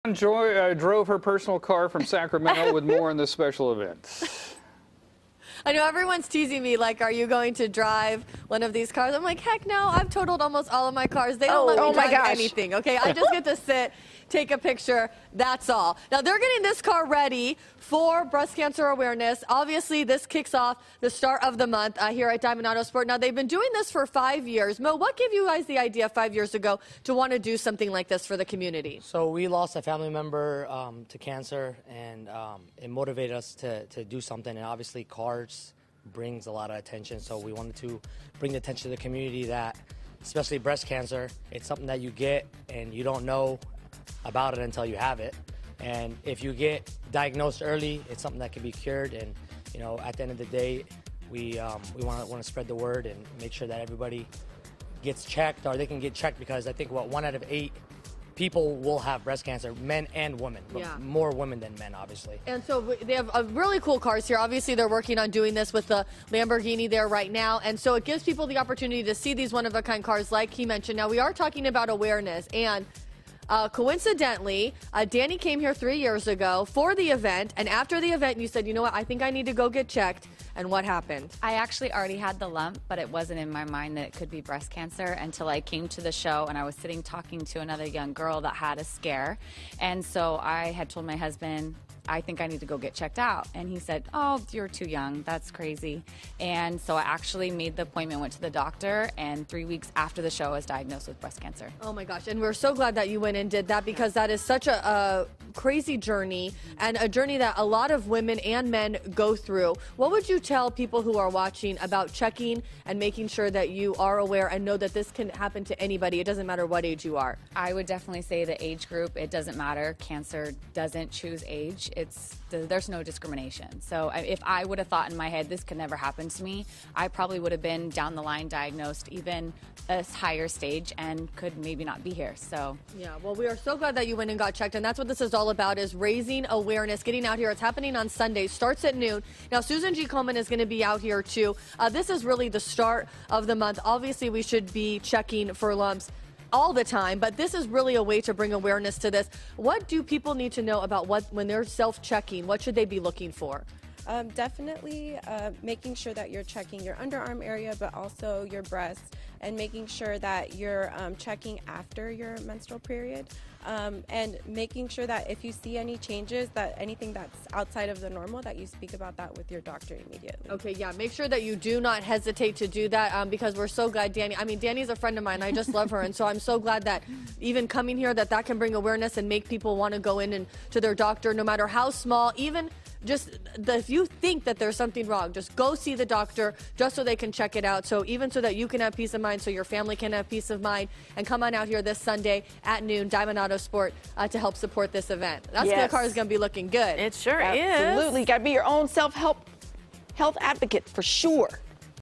Breast Cancer Event
Cambi Brown is talking to a breast cancer survivor and finding out what Diamond Autosport is doing on Sunday to raise money for Breast Cancer Awareness!